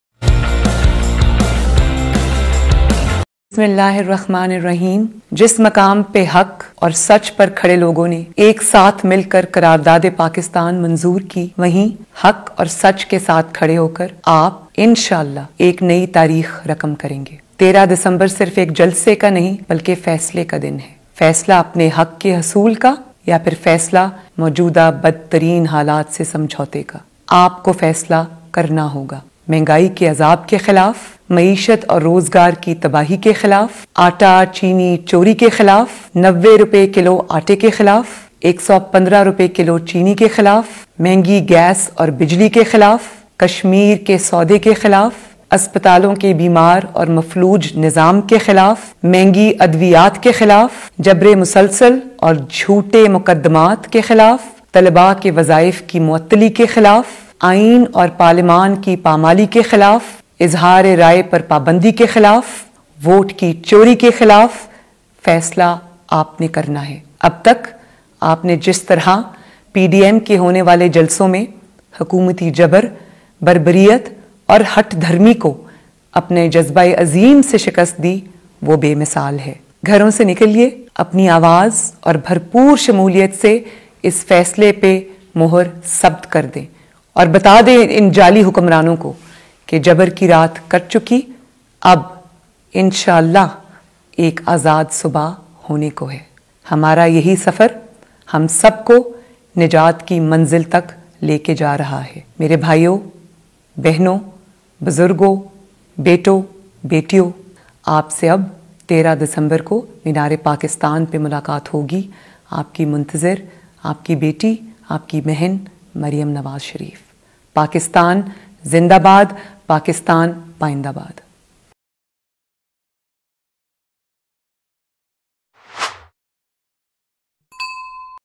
An important video message of Maryam Nawaz came regarding the PDM Lahore jalsa on December 13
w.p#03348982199